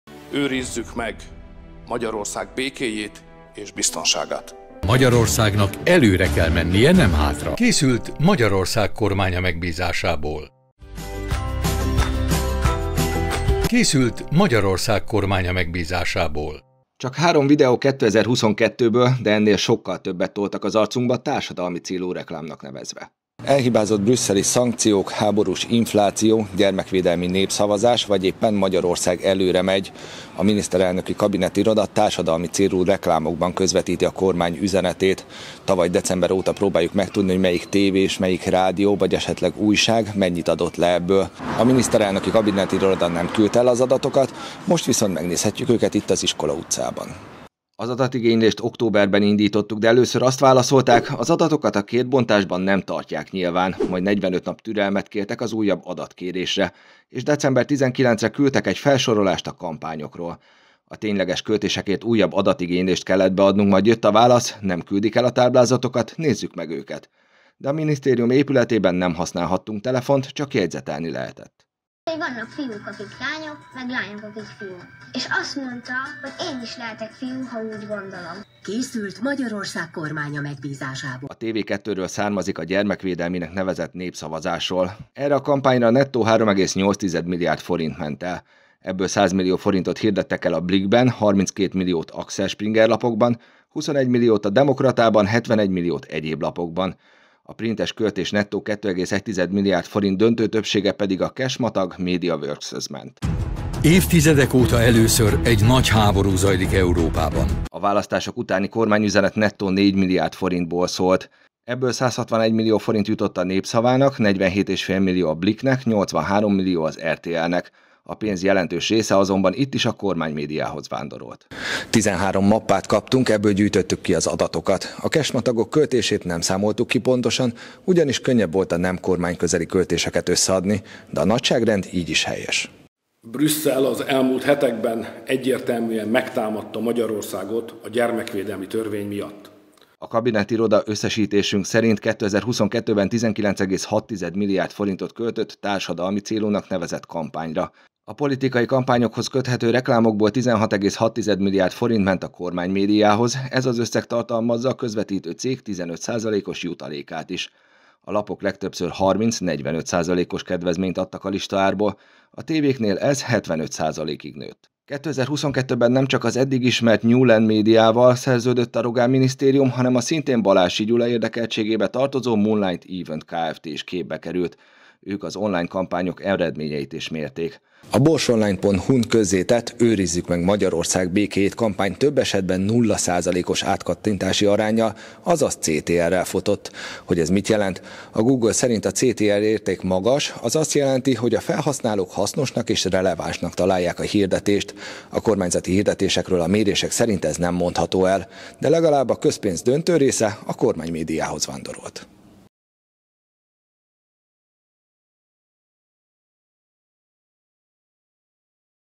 Így vándorolt 16,6 milliárd forint közpénz a kormánymédiához társadalmi célú reklámként
Öt hónapba telt, mire a Rogán Antal vezette Miniszterelnöki Kabinetirodával eljutottunk odáig, hogy megmutatják, 2022-ben a társadalmi célú reklámként futó kampányokra mennyit költöttek, és hol jelentek meg a hirdetések. Az állami milliárdok legnagyobb része a kormányközeli médiánál landolt, de kapott belőle a Blikk, a Népszava, az ATV és az RTL is. Cikkünk a témáról